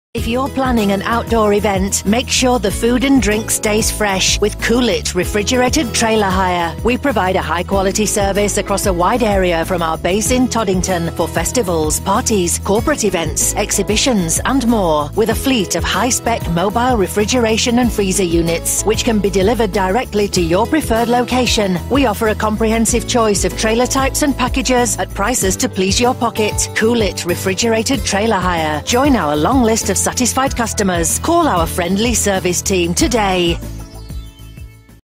Mobile Refrigeration - Coolit Refrigerated Trailer Hire
When you're planning an outdoor event, you don't want to be let down by warm drinks or food that's less than fresh. We have a range of mobile refrigeration units delivering to parties, weddings and events across the country. Keep cool.